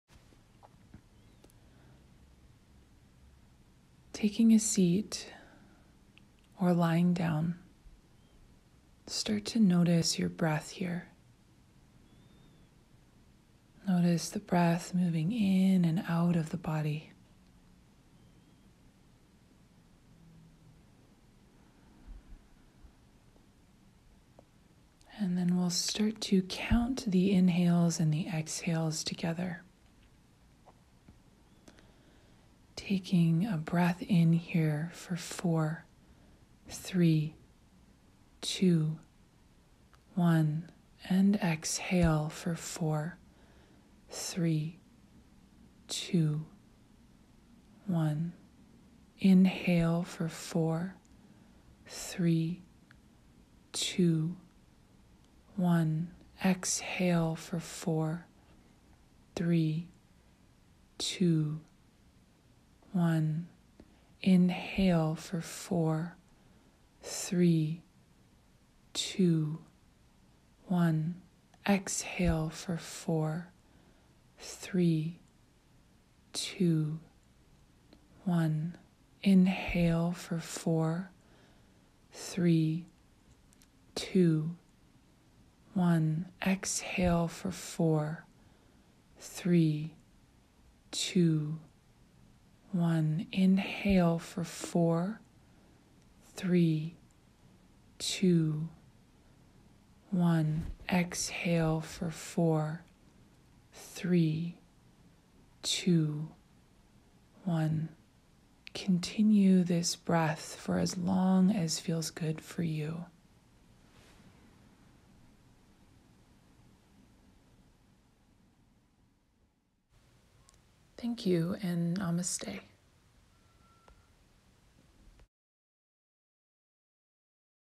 Meditation Reset || Yoga by Melanie Emlyn
Reset your body on all levels, physical, mental and energetic with this simple 2 minute breathwork practice.